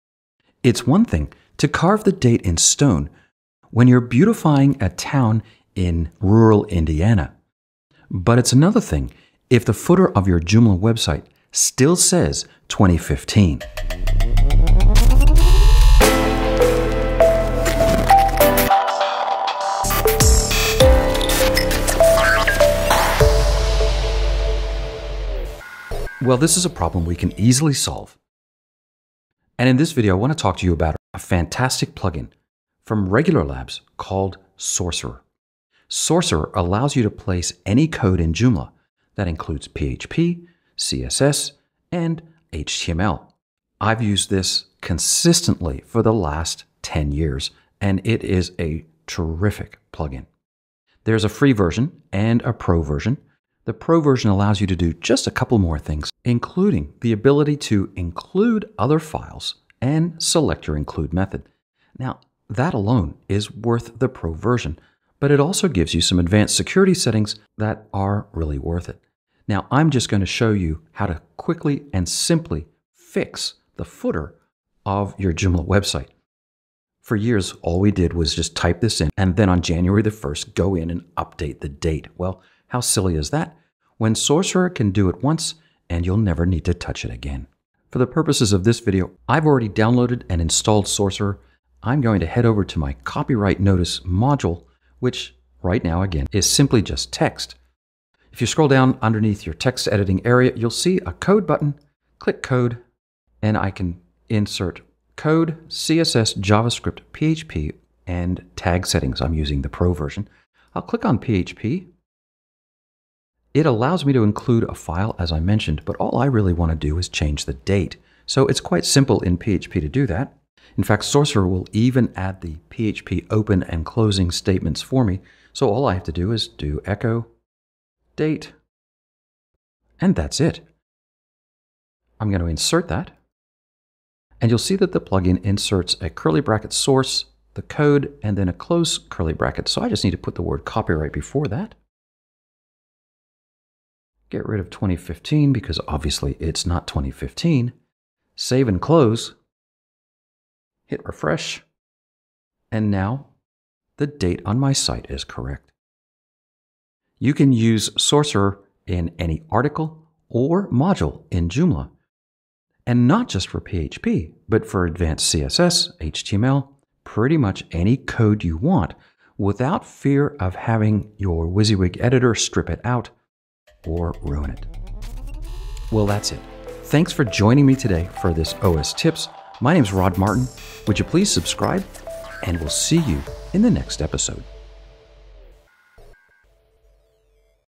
OSTips: Adding Code to Articles and Modules in Joomla
If you've ever tried to add Javascript, PHP, or even CSS to your Joomla article or module, you know the editor will likely strip it out.  Using the Sourcerer plugin from RegularLabs.com solves this problem for you!